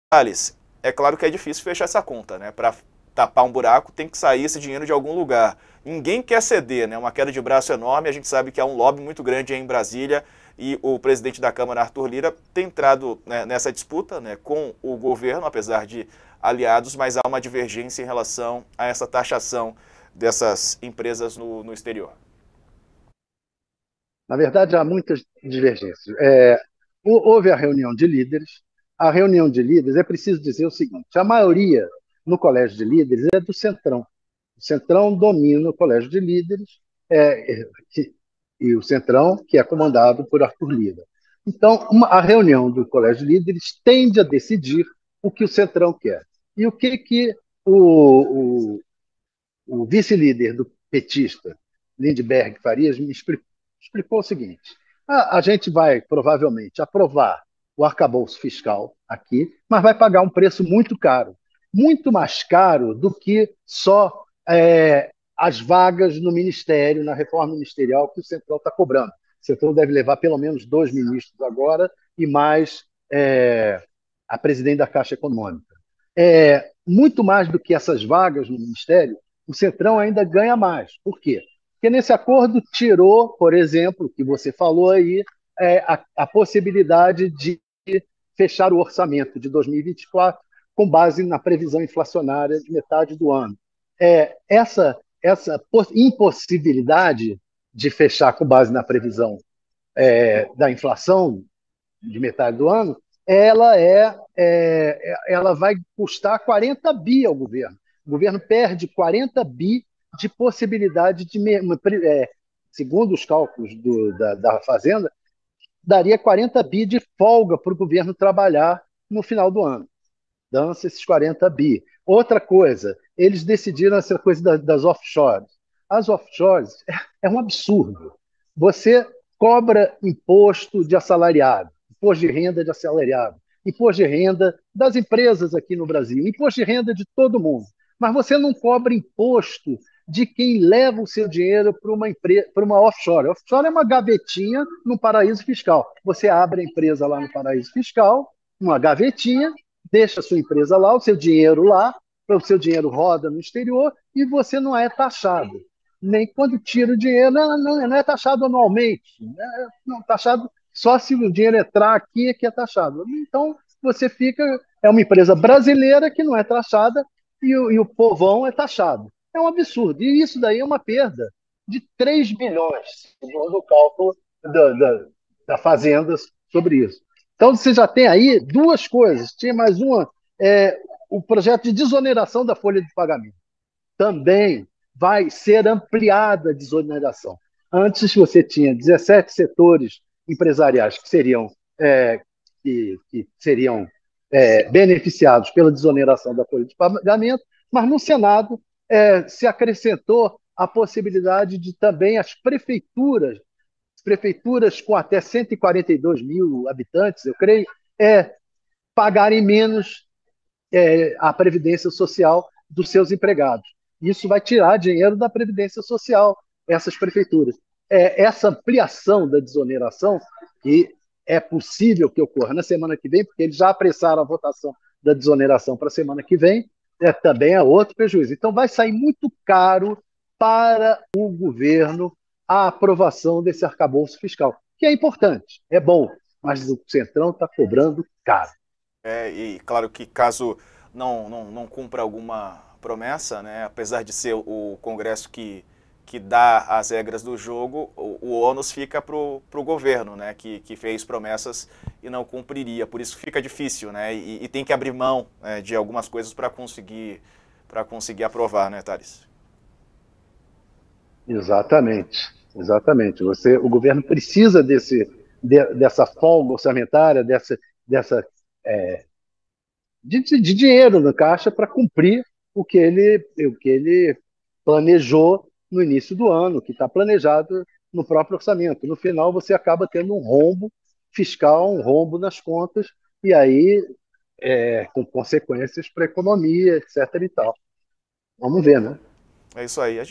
Arcabouço fiscal: 'Centrão cobrou caro pela aprovação da regra fiscal', diz vice-líder petista
A Câmara dos Deputados discute e vota nesta terça-feira (22) as alterações feitas pelo Senado no projeto do Arcabouço Fiscal. No UOL News, o colunista Tales Faria comenta o assunto.